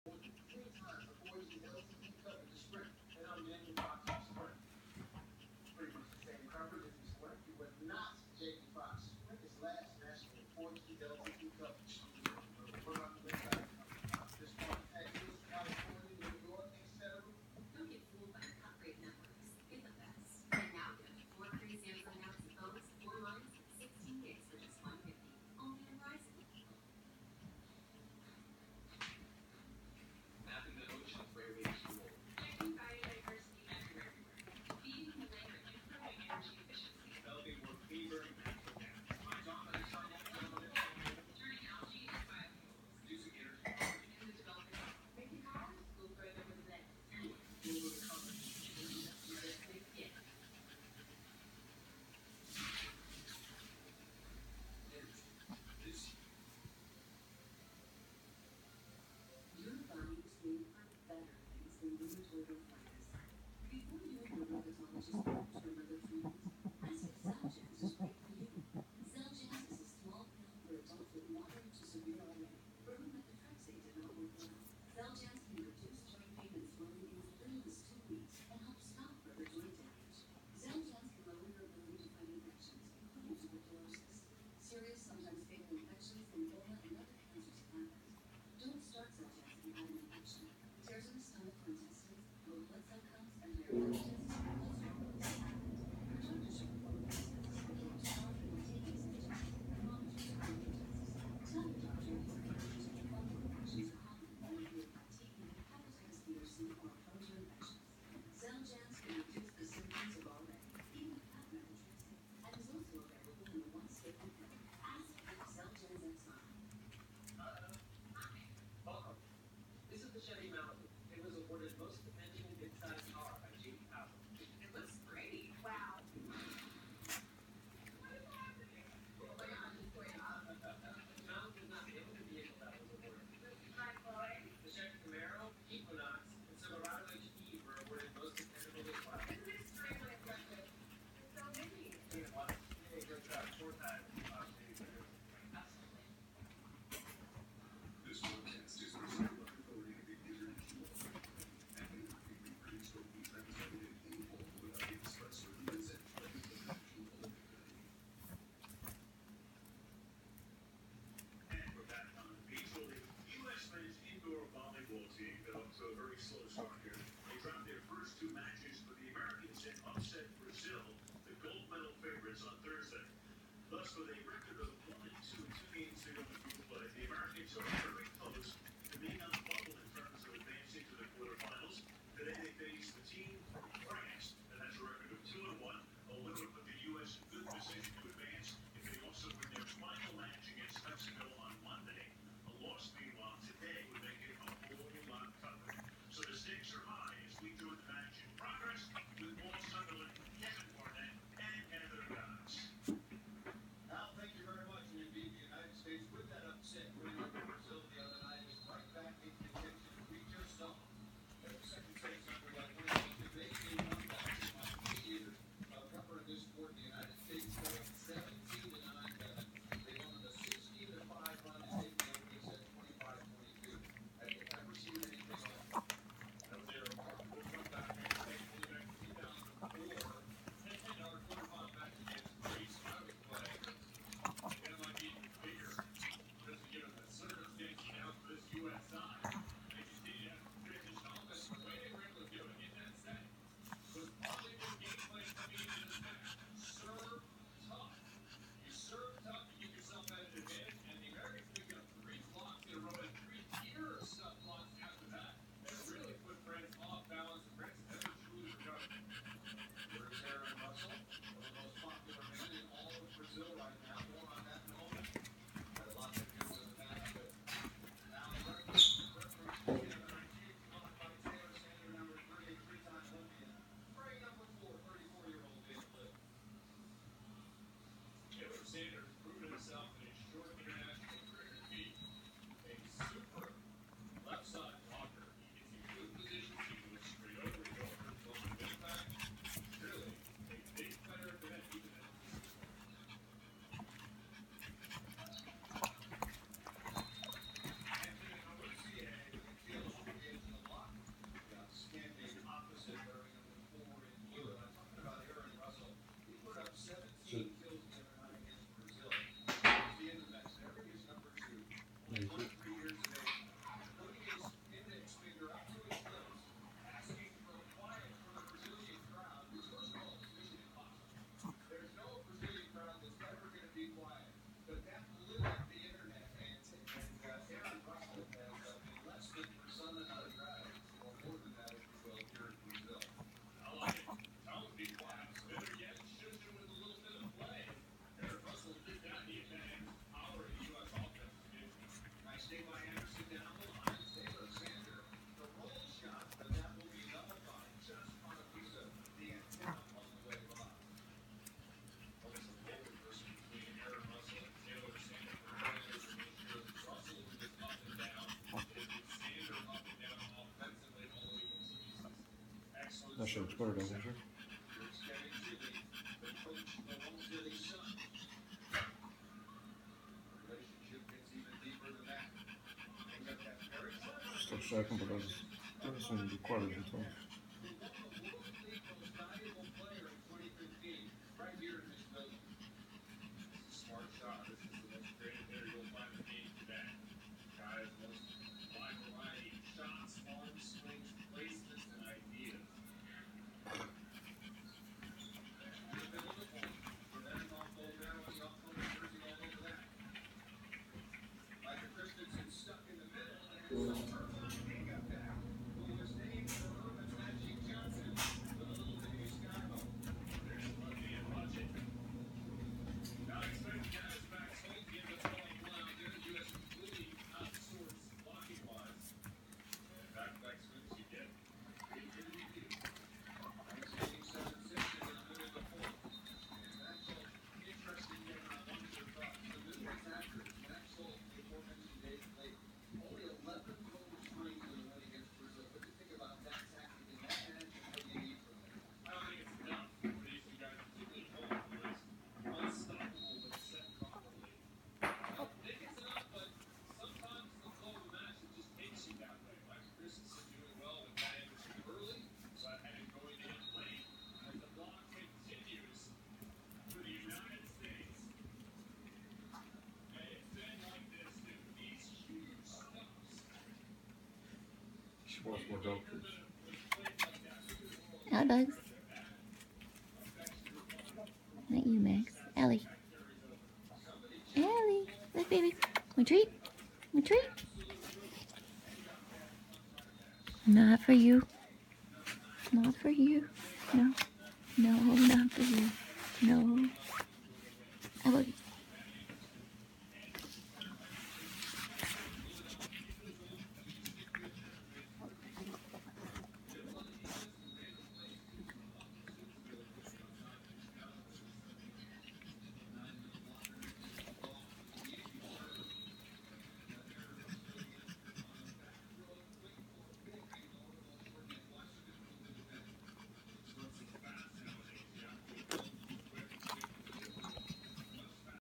Alli's muscle tremors - 8.13.2016 Episode 2
Video shot of Alli's muscle tremors before breathing issues.